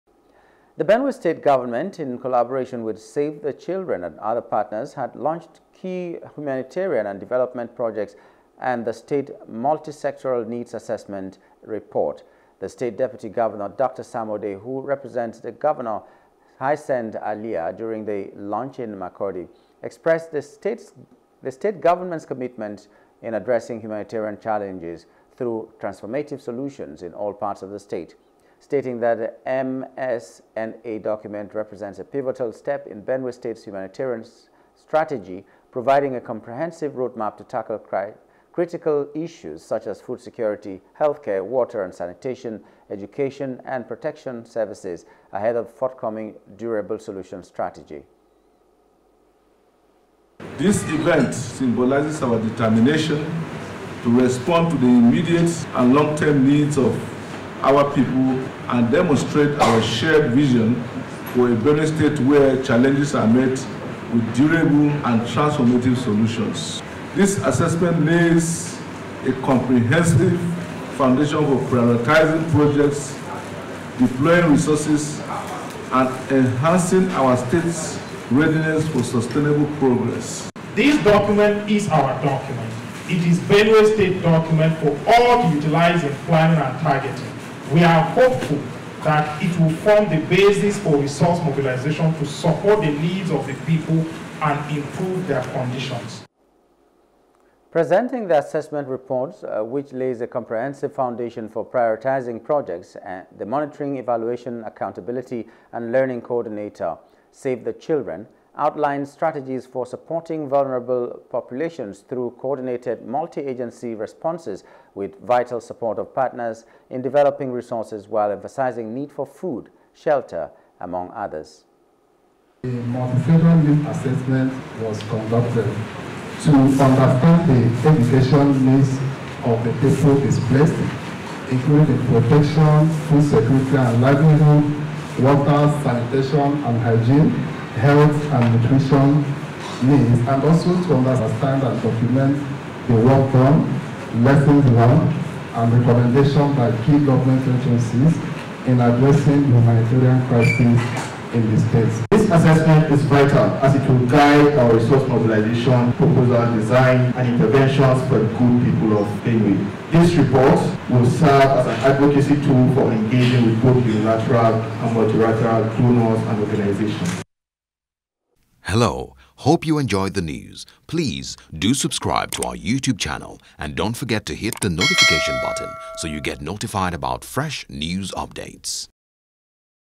Benue: Govt, Partners Unveil Mnsa Report For The Displaced
In collaboration with Save the Children and other partners, the Benue state government has launched Key Humanitarian and Development Projects and the State Multisectoral Needs Assessment(MSNA) report.
The State Deputy Governor, Dr Sam Ode, who represented Governor Hyacinth Alia during the launch in Makurdi, expressed the state government's commitment to addressing humanitarian challenges through transformative solutions in all parts of the state.
Stating that the MSNA document represents a pivotal step in Benue State’s humanitarian strategy, providing a comprehensive roadmap to tackle critical issues such as food security, healthcare, water and sanitation, education, and protection services ahead of the forthcoming Durable Solutions Strategy.